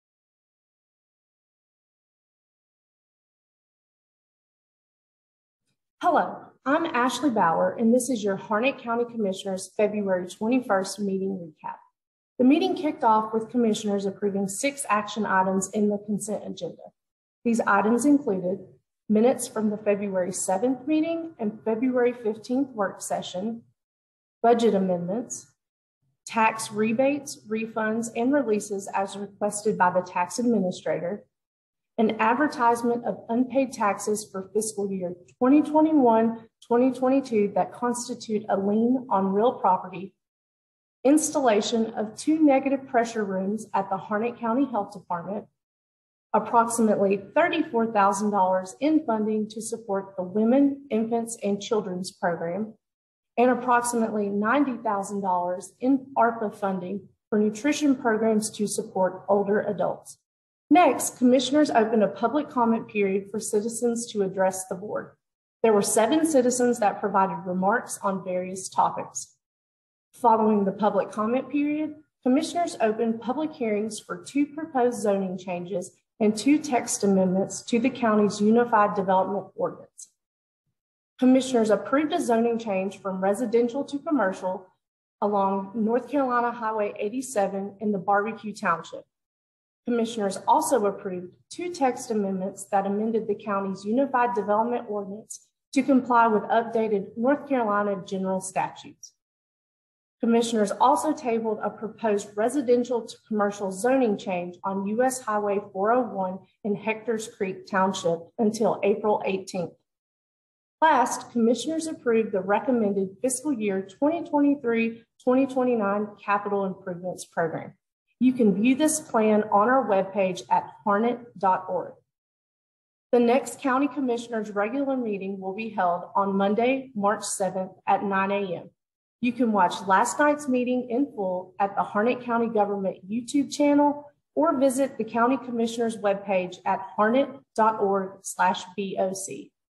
February 21, 2022 County Commissioners Meeting Recap
Here's a quick recap for the Harnett County Board of Commissioners February 21, 2022 Meeting.